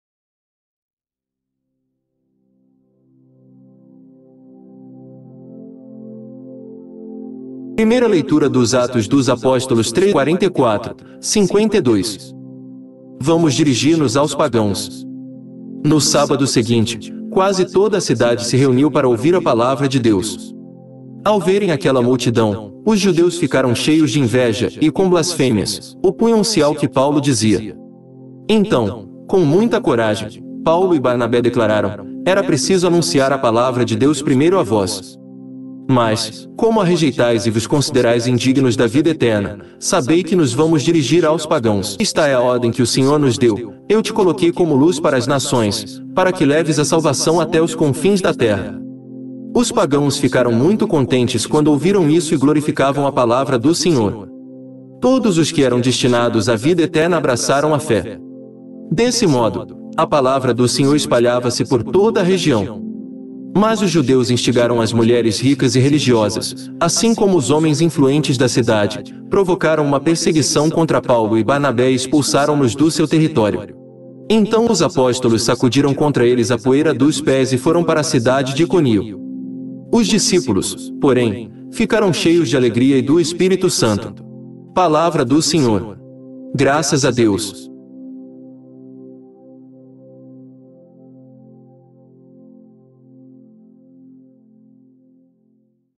Primeira Leitura dos Atos dos Apóstolos 13,44-52
44. No sábado seguinte, quase toda a cidade se reuniu para ouvir a Palavra de Deus.

45. Ao verem aquela multidão, os judeus ficaram cheios de inveja e, com blasfêmias, opunham-se ao que Paulo dizia.

46. Então, com muita coragem, Paulo e Barnabé declararam: “Era preciso anunciar a Palavra de Deus primeiro a vós. Mas, como a rejeitais e vos considerais indignos da vida eterna, sabei que nos vamos dirigir aos pagãos.

47. Porque esta é a ordem que o Senhor nos deu: ‘Eu te coloquei como luz para as nações, para que leves a salvação até os confins da terra'”.

48. Os pagãos ficaram muito contentes quando ouviram isso e glorificavam a Palavra do Senhor. Todos os que eram destinados à vida eterna abraçaram a fé.

49. Desse modo, a Palavra do Senhor espalhava-se por toda a região.

50. Mas os judeus instigaram as mulheres ricas e religiosas, assim como os homens influentes da cidade, provocaram uma perseguição contra Paulo e Barnabé e expulsaram-nos do seu território.

51. Então os apóstolos sacudiram contra eles a poeira dos pés e foram para a cidade de Icônio.

52. Os discípulos, porém, ficaram cheios de alegria e do Espírito Santo.